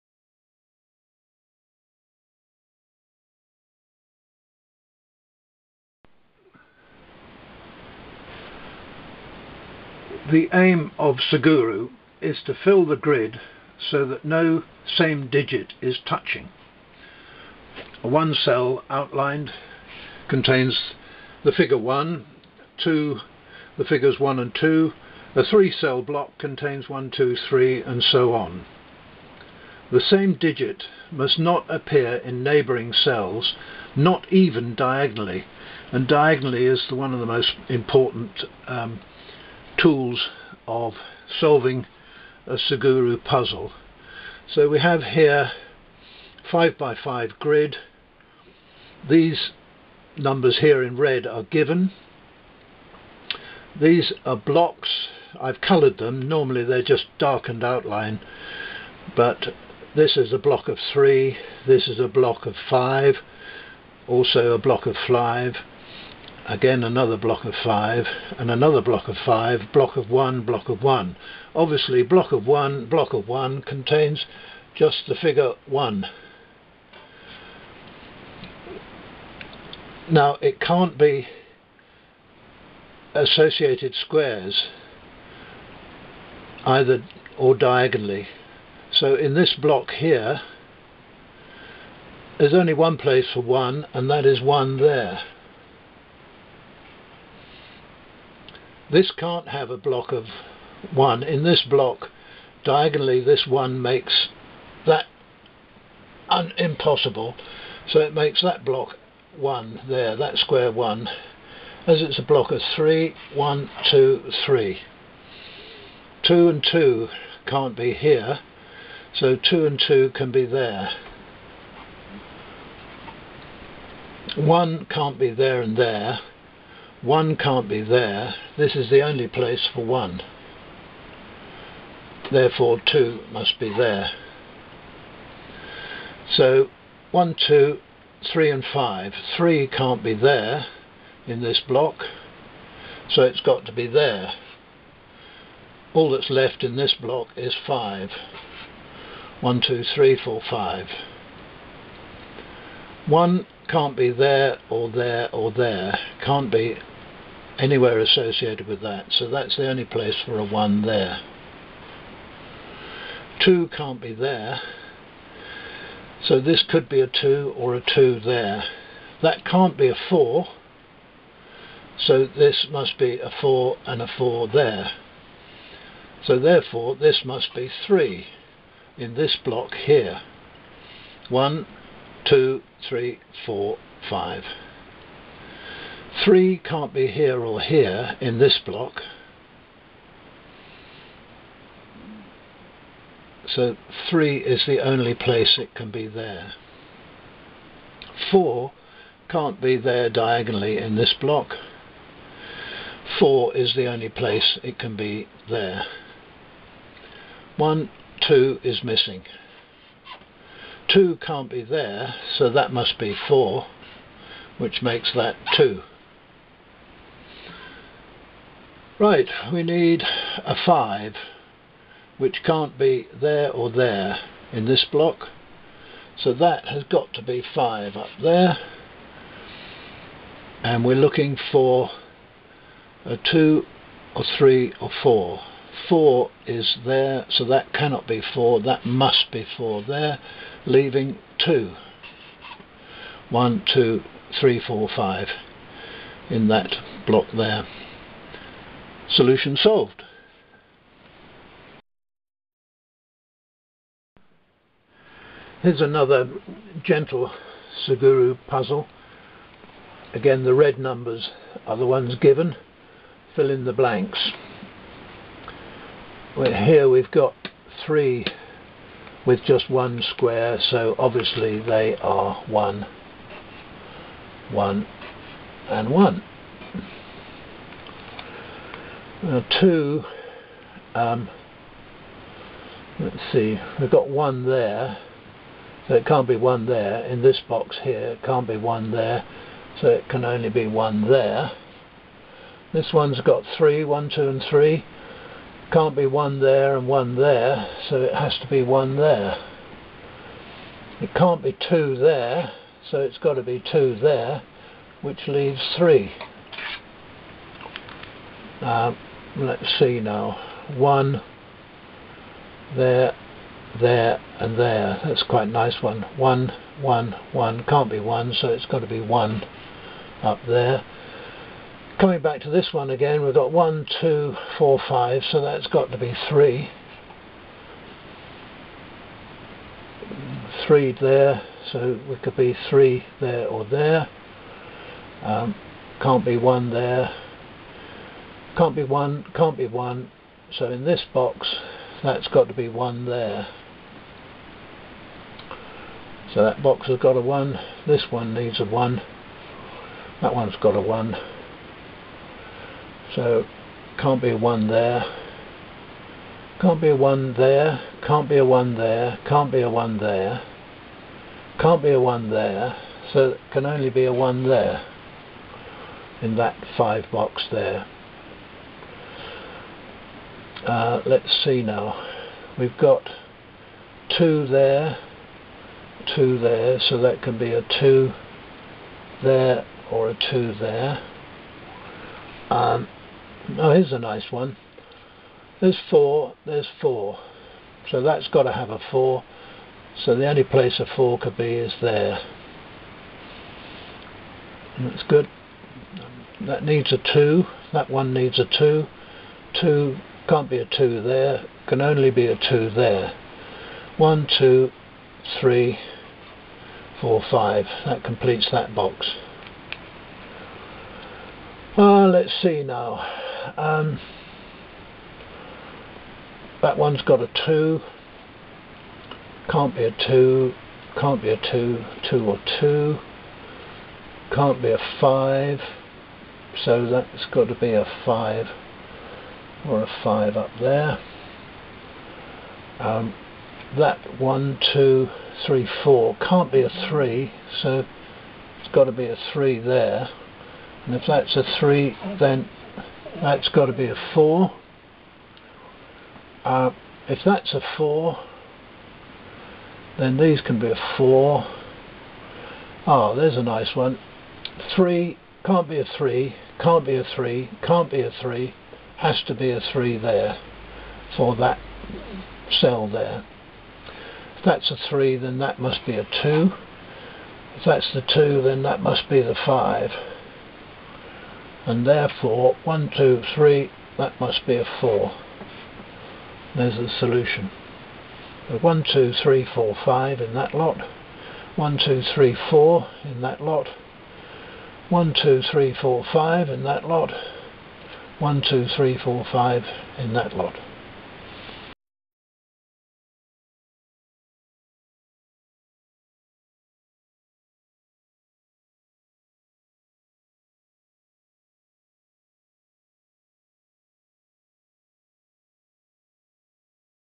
Solving Suguru Puzzles
How I solved some gentle suguru puzzles that have just started to appear on the back page of the Daily Telegraph newspaper.
Some other puzzle solutions are -